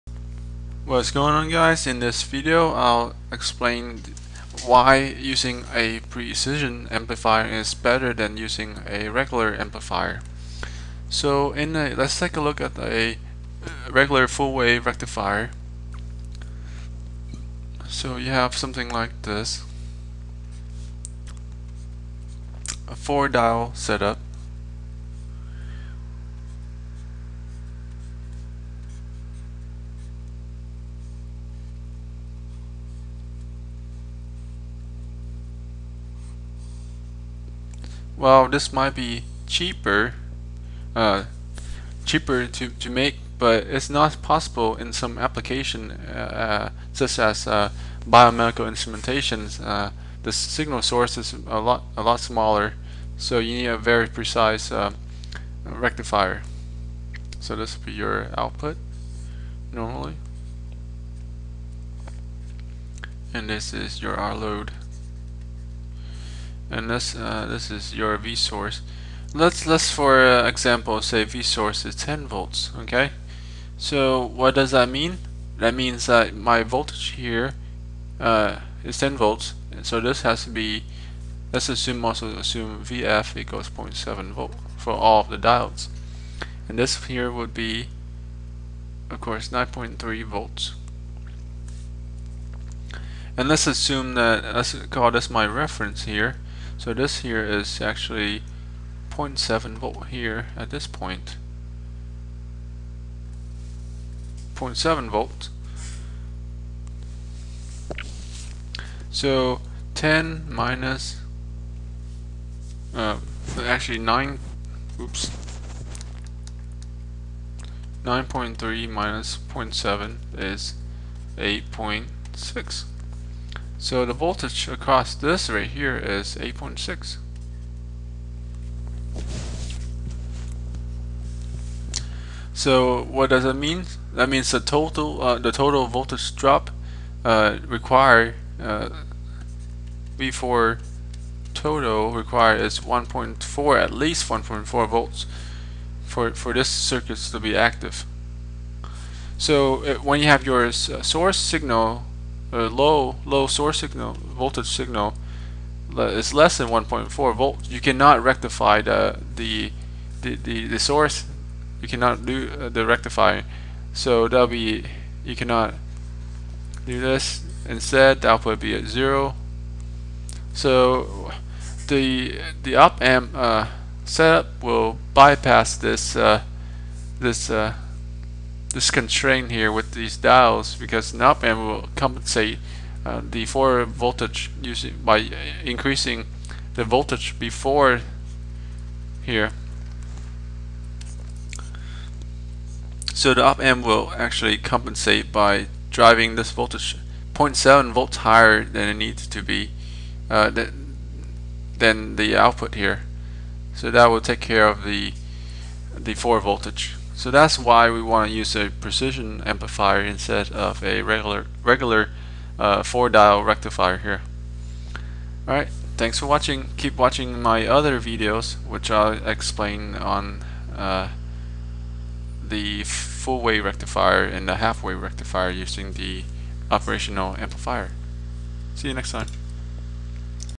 Regular Versus Precise Full Wave Rectifier Part 1
Why use a precision rectifier? I will explain in this video.